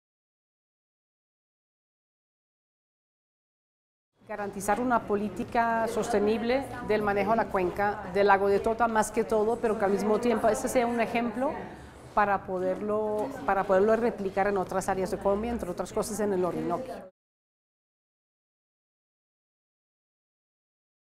Recuperación Lago de Tota
Declaraciones de la embajadora de la Unión Europea, Tanya Van Gool.